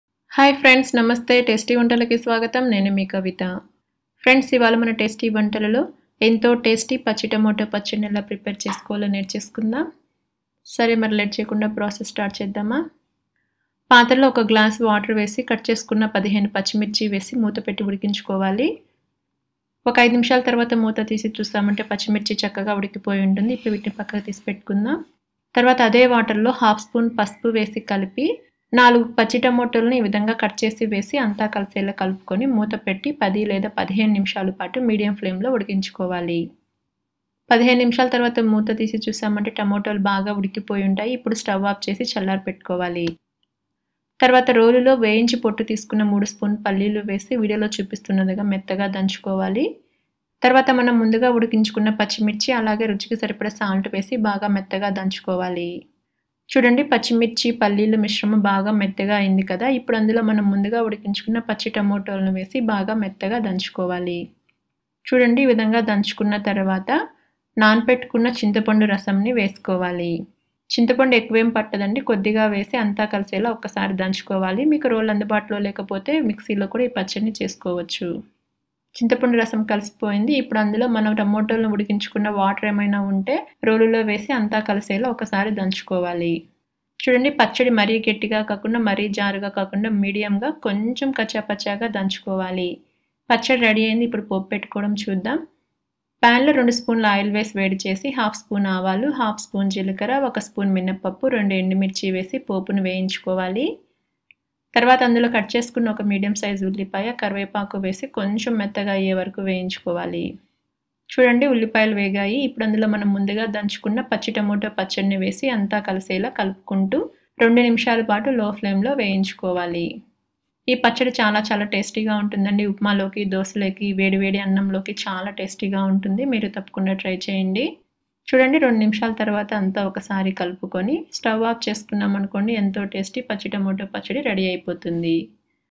Pachi Tomato roti pachadi in Telugu | Green Tomato chutney for rice idli and dosa
Learn how to prepare tasty chutney with green tomatoes. This will go well with Idli, dosa, upma and rice.